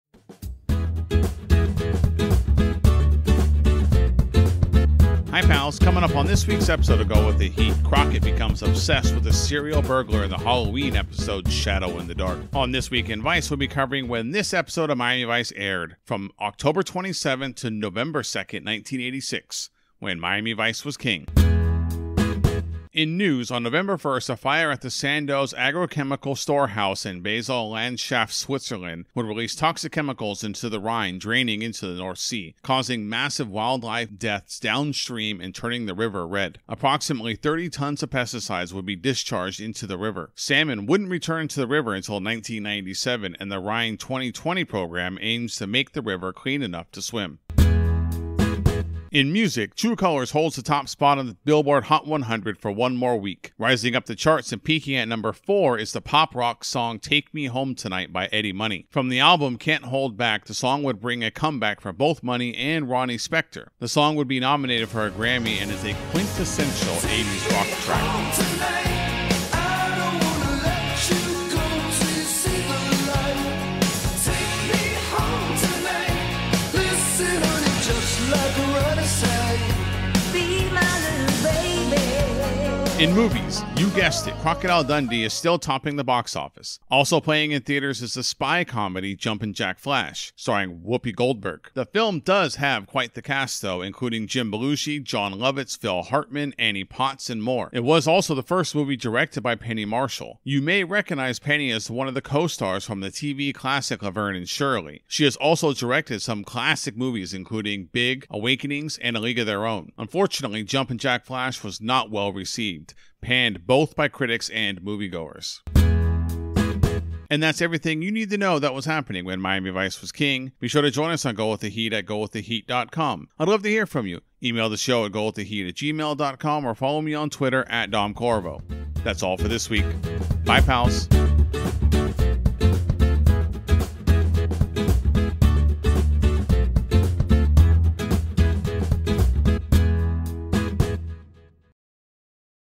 This Week In Vice 40 – Oct. 27 – Nov. 2, 1986
Hi pals, coming up on this weeks episode of Go With The Heat Crockett becomes obsessed with a serial burglar in the Halloween episode Shadow in the Dark. On This Week in Vice we'll be covering the when this episode aired, from Oct. 27 – Nov. 2, 1986, when Miami Vice was King.

Original art provided by:
Jenna Barham (@jennaabarham)